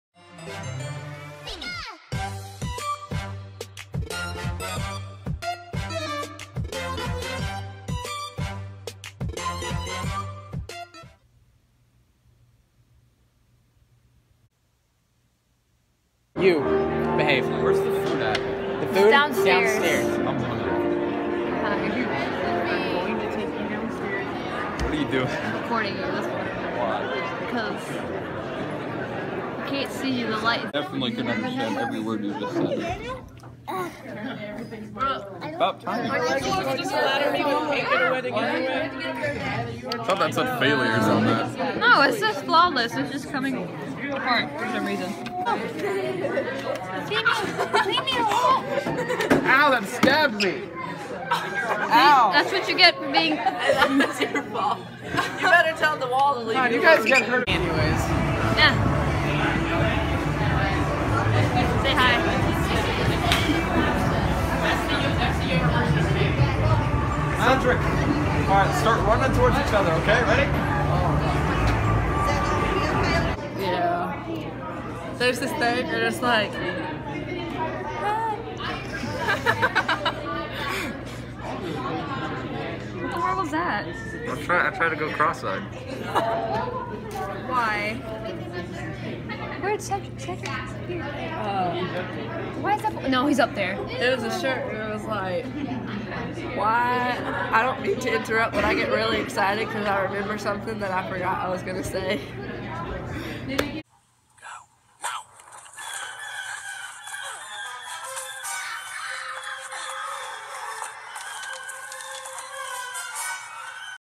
Sunday Vlog | Leah
I would've recorded us skating but they don't allow it, enjoy us being goofs!